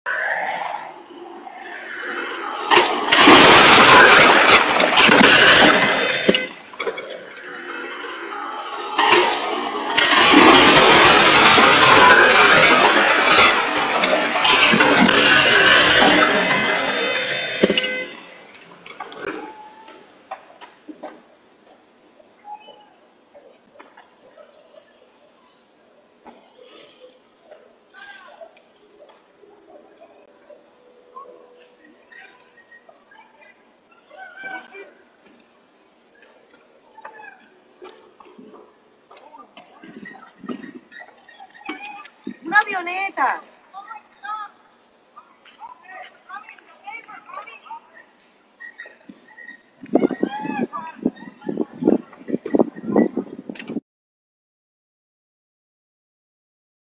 Miami, aereo precipita sulla strada e colpisce un Suv
Nell’incidente sono morte le due persone a bordo del velivolo, mentre madre e figlio che viaggiavano sull’auto sono rimasti feriti